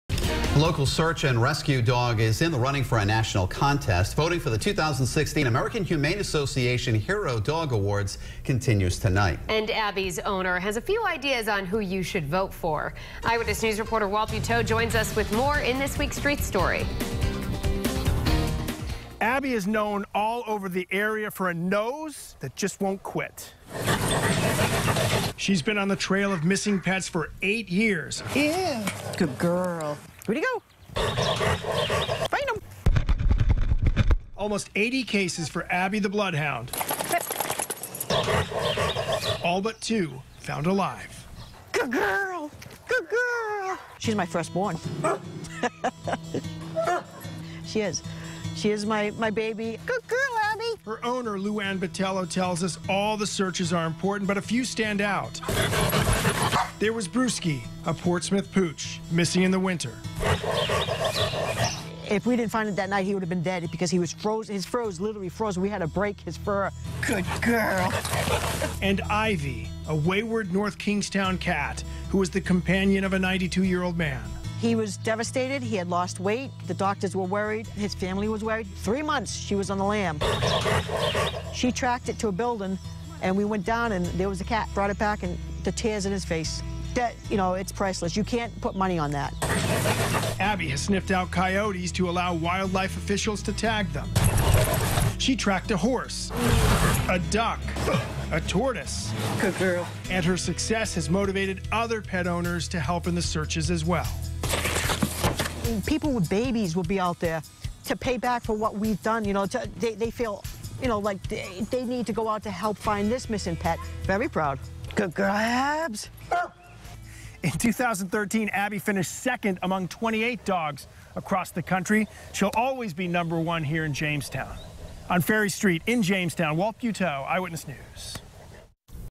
Abby is known all over the area for a nose that just won't quit
A local search and rescue dog is in the running for a national contest. Voting for the 2016 American Humane Association Hero Dog Awards continues Friday night.